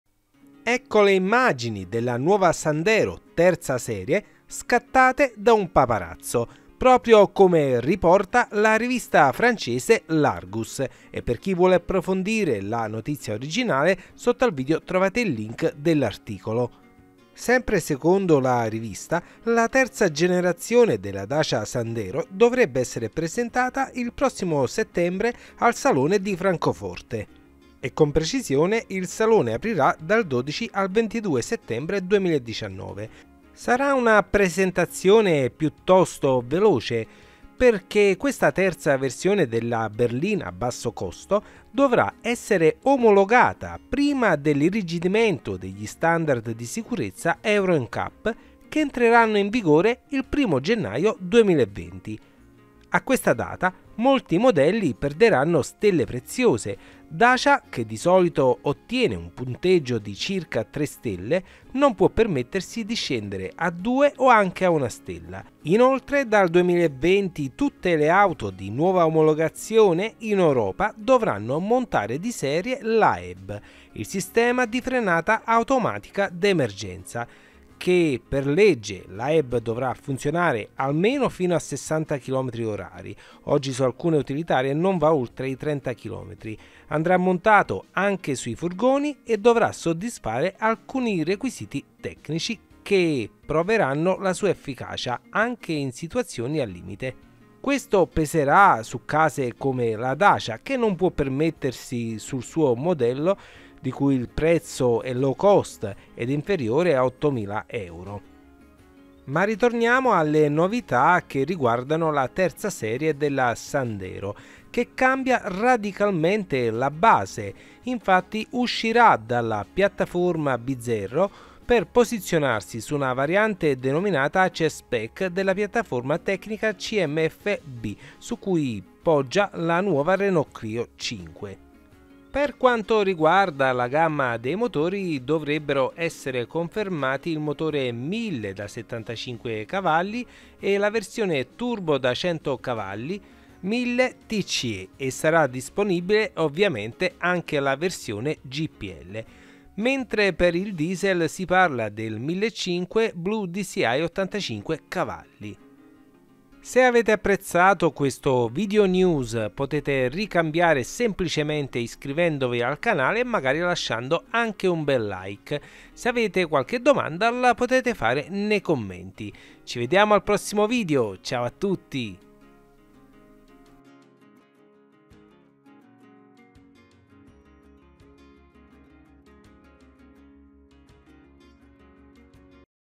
Nuova Dacia Sandero 3 foto spia presentazione Francoforte
Nel video trovi le prime immagini della nuova Dacia Sandero 3 che verrà presentata al salone di Francoforte secondo la rivista Francese L'argus che trovi qui --) a settembre 2019 in anteprima ma che verrà commercializzata nel 2020 questa nuova dacia sandero terza serie prodotta con un diverso pianale e una nuova gamma di motori nel video ti parlo delle novità.